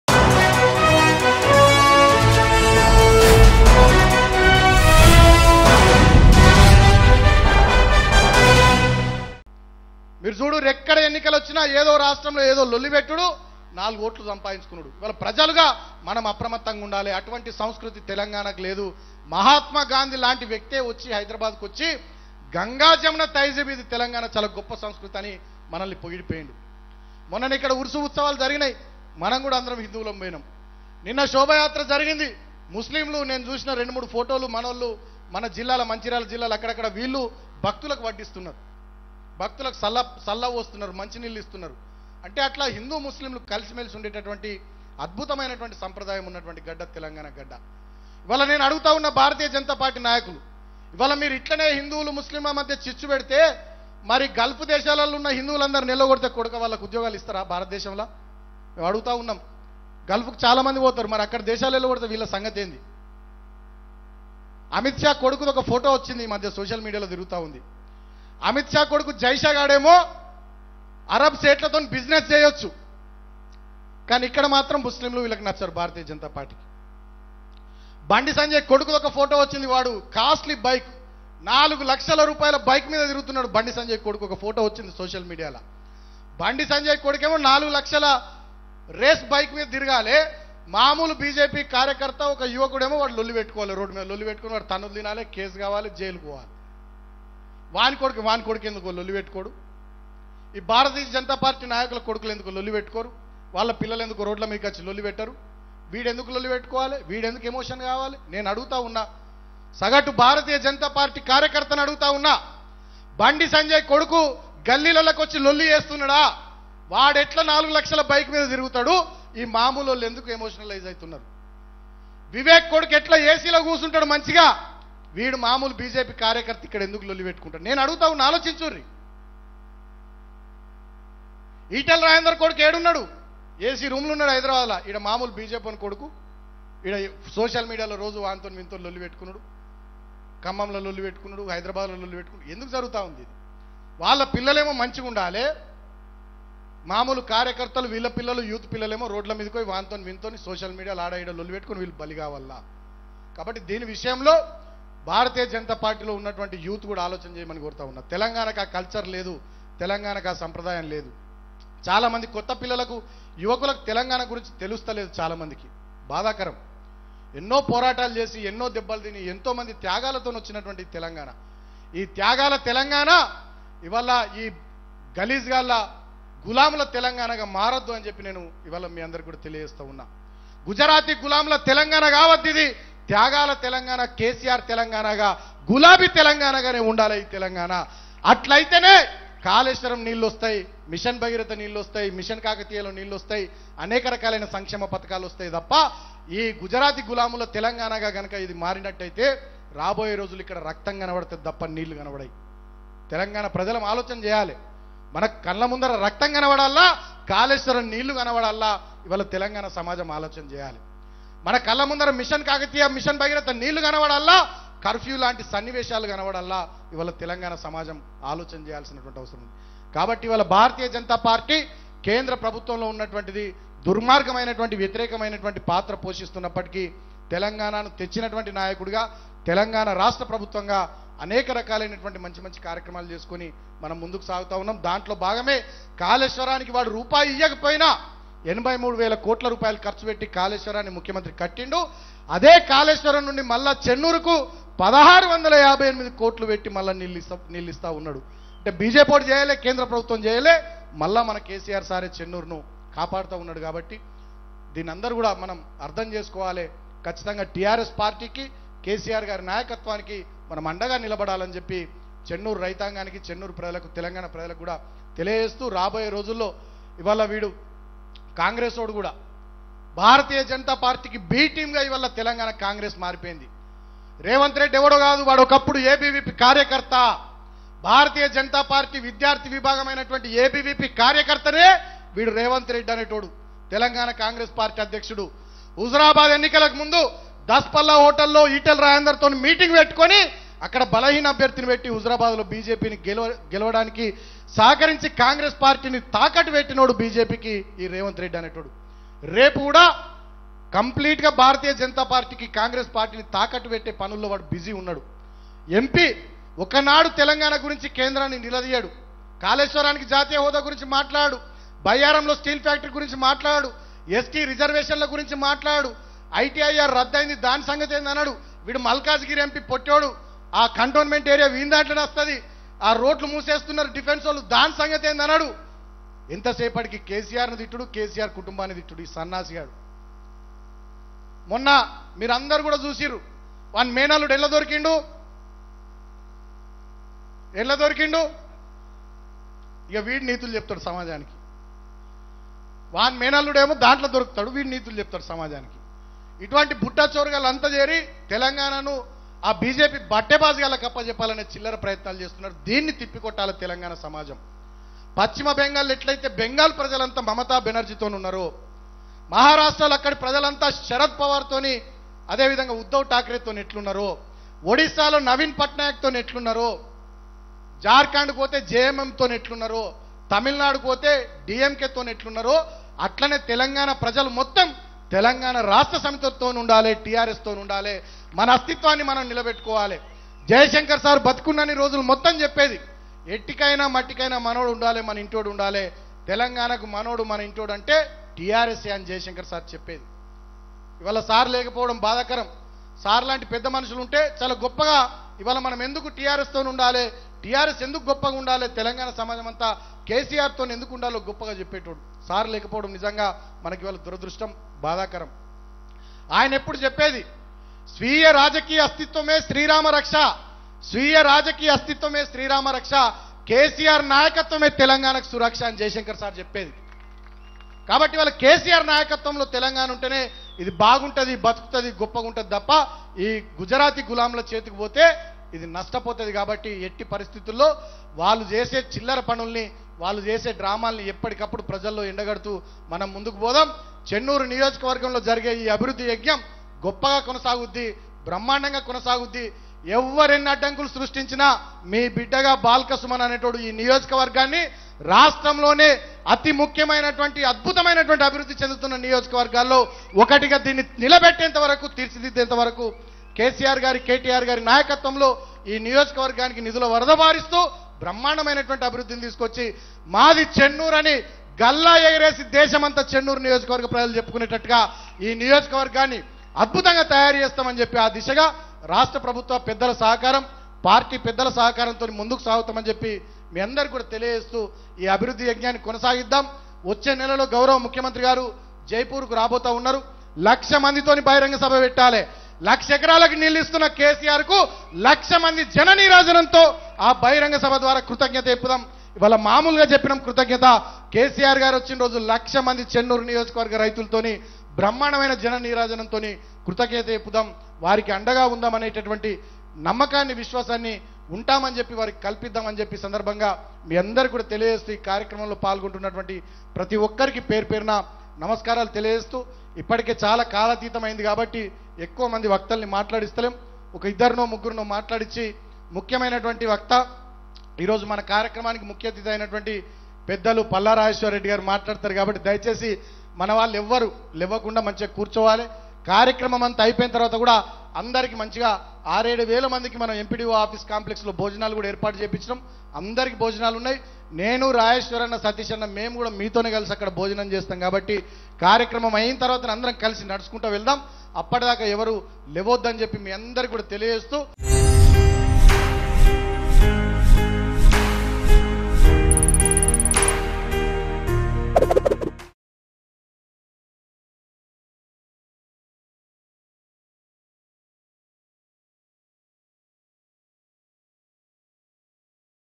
TRS MLA Balka Suman Serious Comments On TPCC Chief Revanth Reddy | Telangana Politics | Mango News
TRS MLA Balka Suman Serious Comments On TPCC Chief Revanth Reddy.